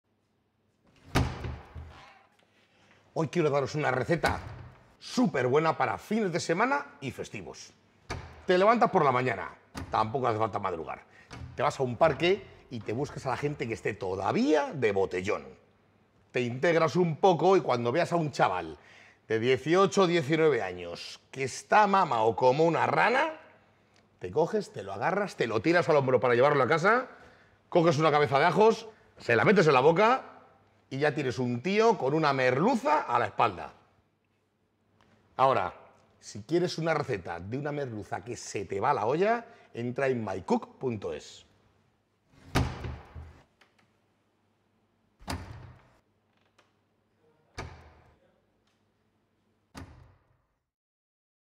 Chicote con Mycook: Receta de Merluza
Una de las recetas "locas" del chef Chicote de esos momentos en los que "se le va la olla".   Si a ti también se te va la olla, participa en nuestro concurso. Podrás ganar una Clase Magistral con Chicote. Y una Mycook.